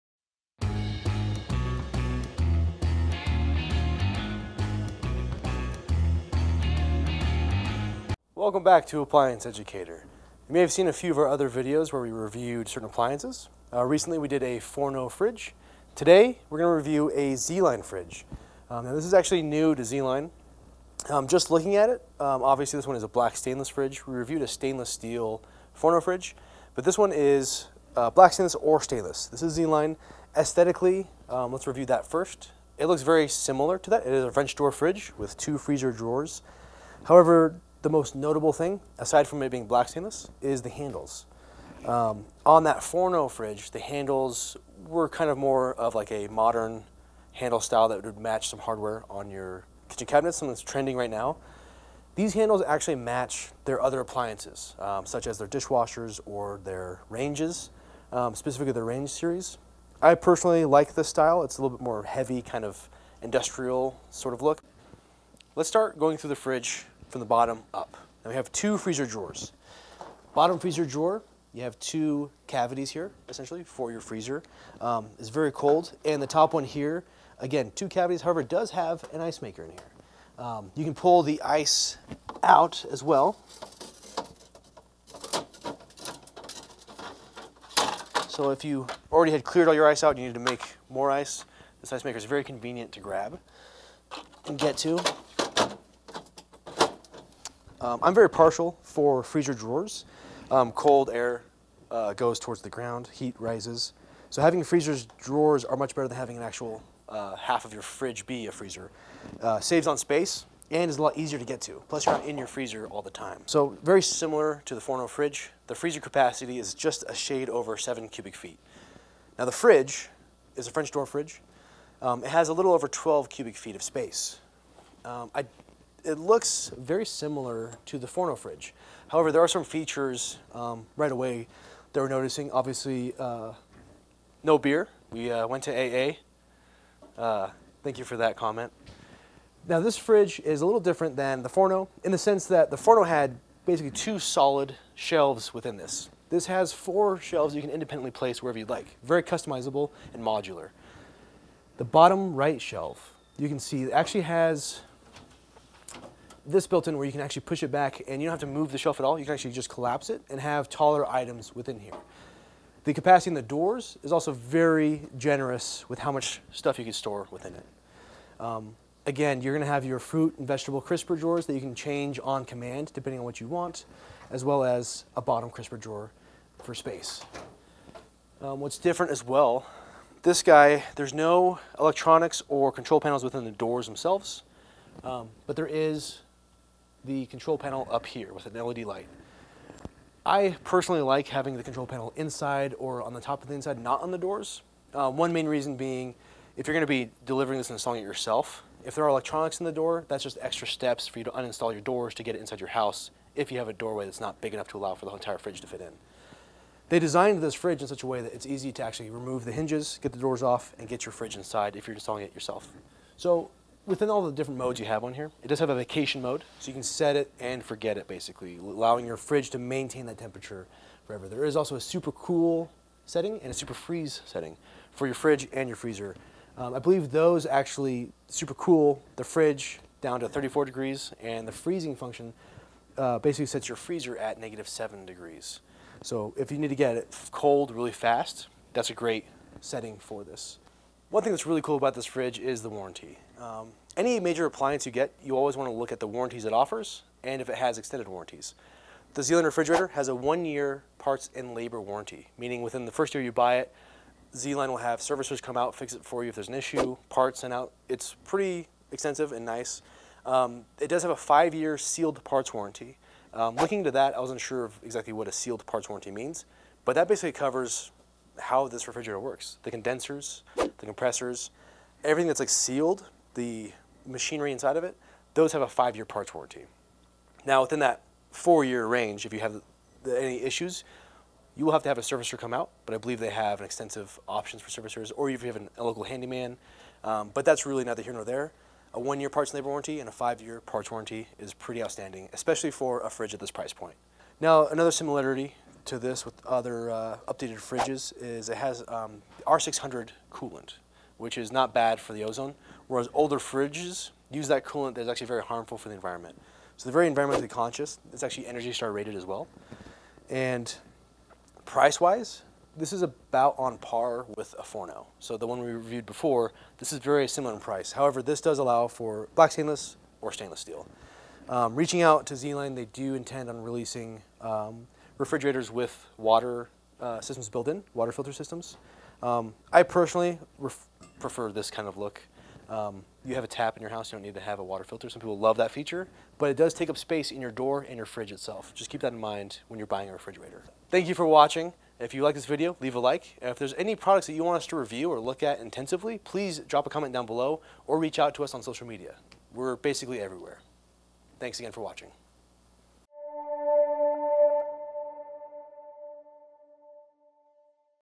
ZLINE Refrigerator Review | AE Before You Buy
It’s no secret that we’ve been a fan of ZLINE and their appliances for the past few years. They’re always releasing new appliances to help you complete your dream kitchen, and their newest release is no exception.

ZLINE Refrigeration has been highly anticipated, and we were ecstatic when we got the opportunity to review it. Here’s what we have to say about the newest appliance from ZLINE.

Chapters:
0:00 Intro
0:30 What we first noticed
1:10 The bottom freezer drawers
2:00 French door fridge
4:00 Warranties
5:00 Final thoughts

What would you like to see us review next?

Explore our AE Blog Series, providing helpful tips and tricks for everything bath and kitchen